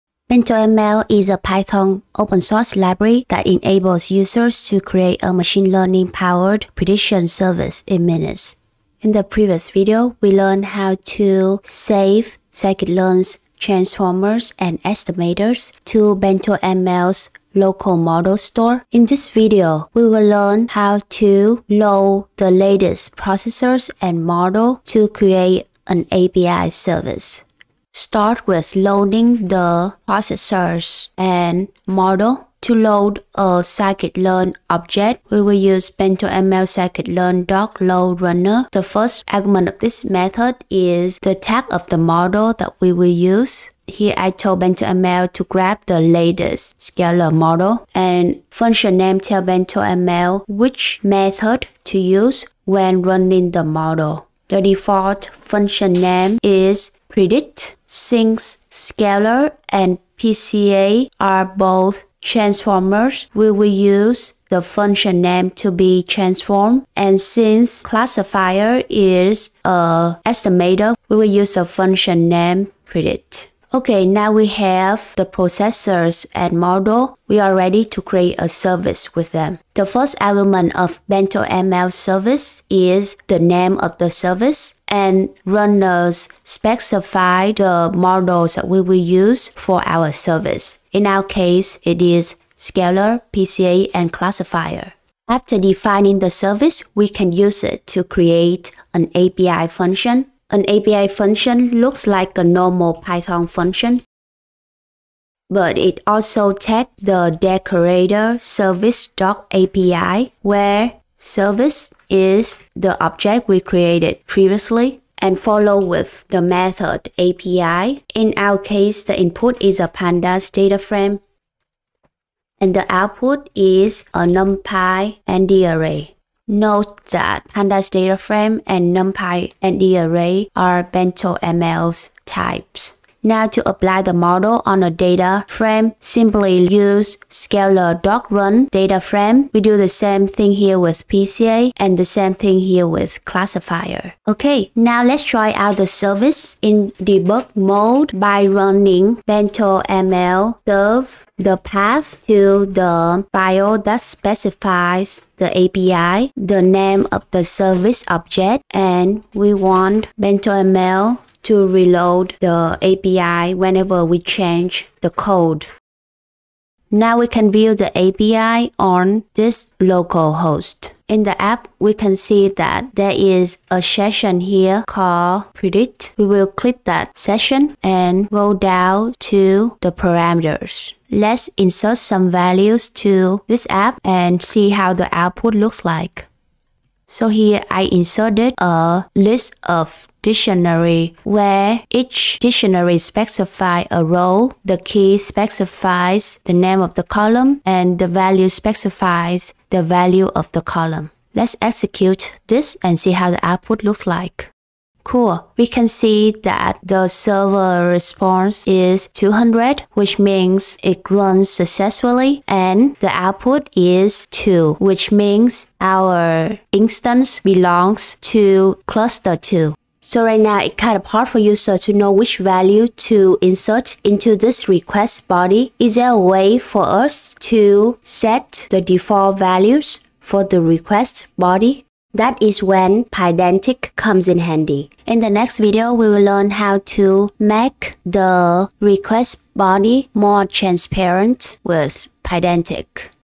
BentoML: Create a Machine Learning Powered API in a Few Lines of Code
BentoML is a Python open-source library that enables users to create a machine learning-powered prediction service in minutes, which helps to bridge the gap between data science and DevOps.

In this video, you will learn how to create an API service in a few lines of code.